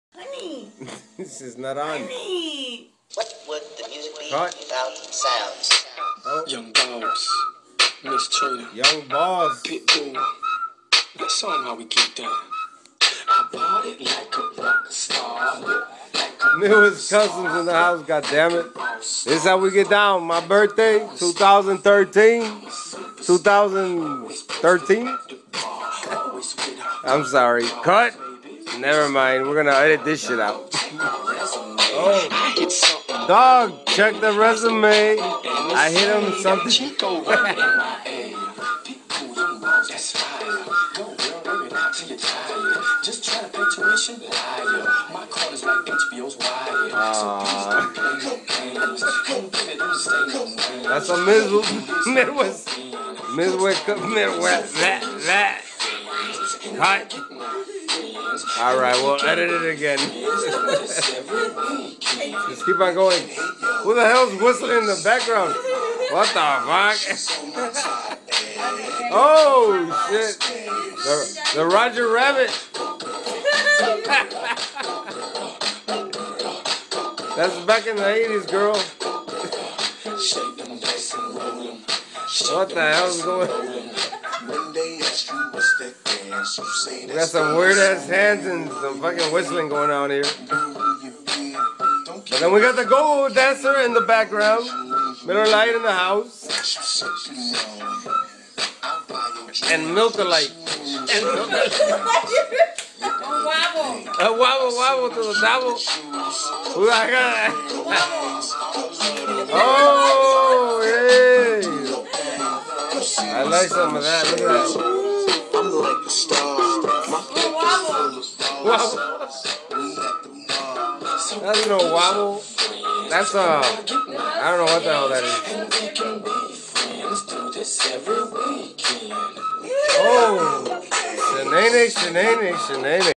My birthday party with my Fam!
Thanx 2 everybody that showed me love yesterday it will not b forgotten! N who's whistling in the background!?!? Lol